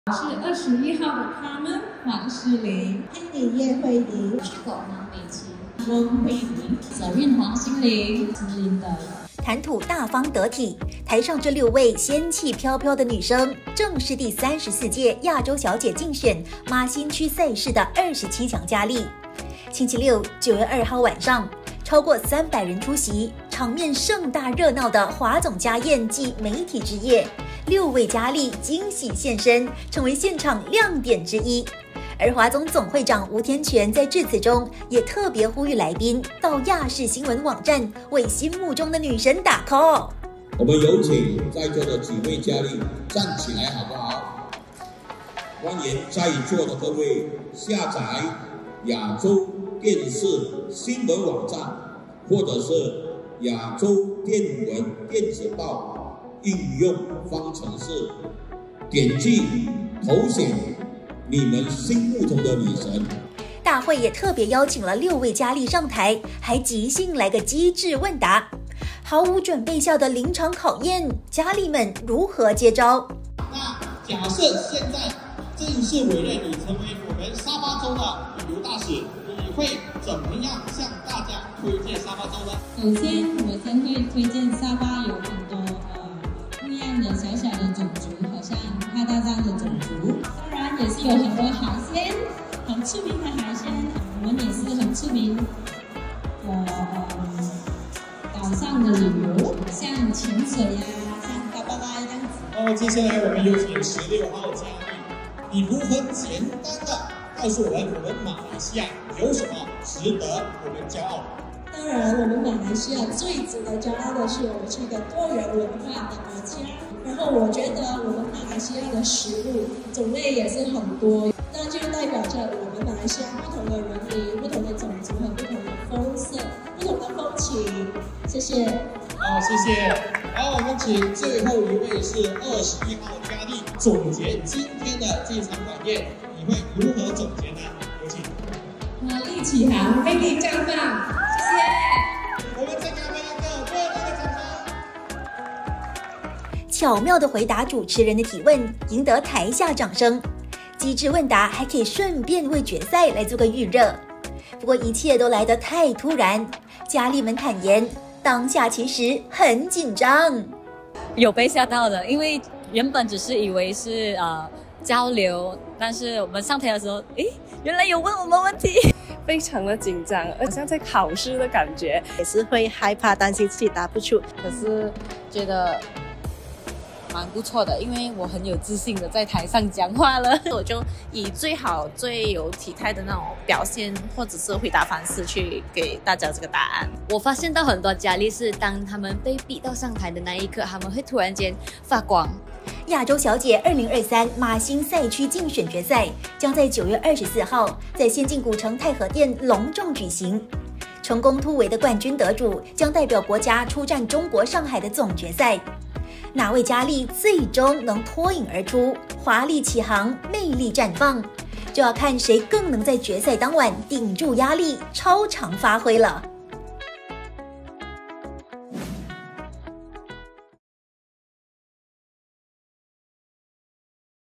现身华总媒体夜落落大方 亚姐竞选佳丽才貌双全
华总家宴暨媒体之夜星期六晚上盛大举行，6位第34届亚洲小姐竞选马新区赛事的27强佳丽也惊喜现身，并被邀请上台来个即席机智问答。毫无准备下的临场考验，佳丽们如何接招？